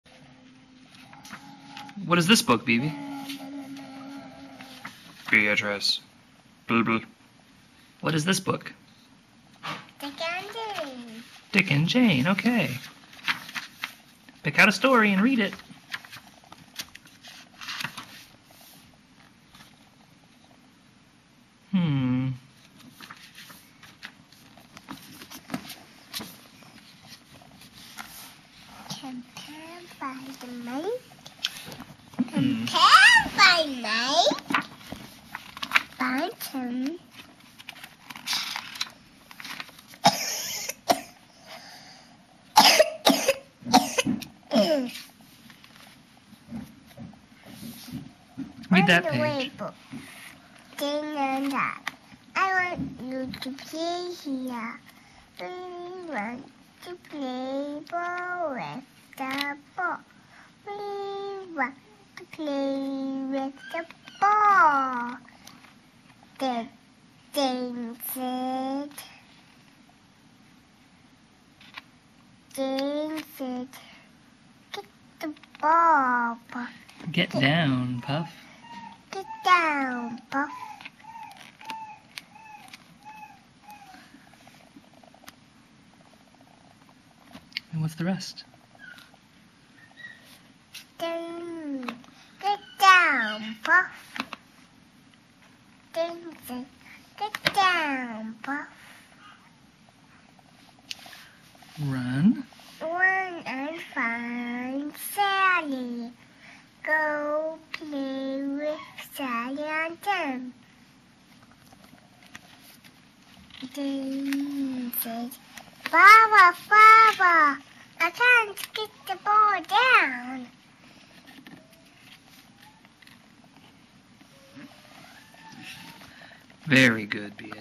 Bebe reads Dick & Jane
We finally caught her on video *reading* this brand new book. We had not read this book to her, so these words are all from her own little brain. Can you believe it?! Not even 3 yet and she's reading. Take that! "My Baby Can Read!"  Who needs your overpriced memorization tools?!